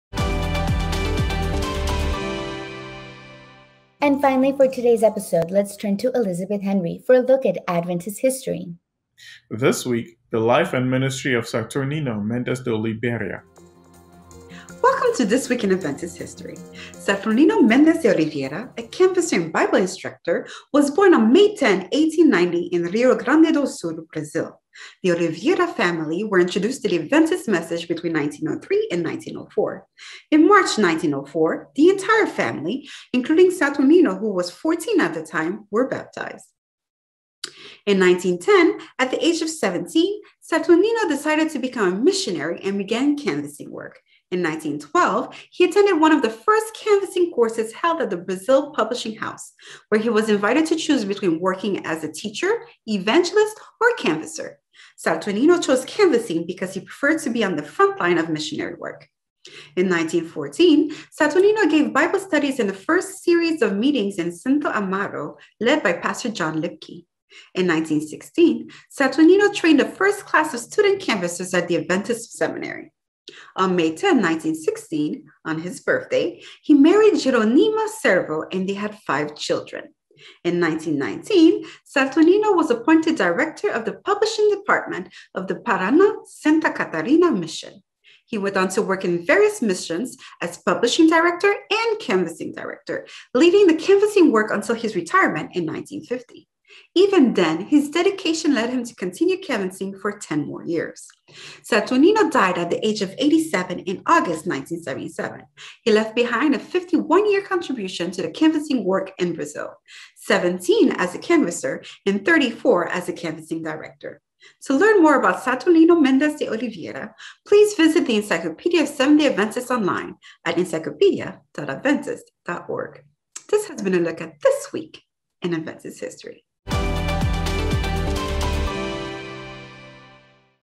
This Week in Adventist History (May 14, 2021)
This week, let's learn about Saturnino Mendes de Oliveria's life of service in Brazil.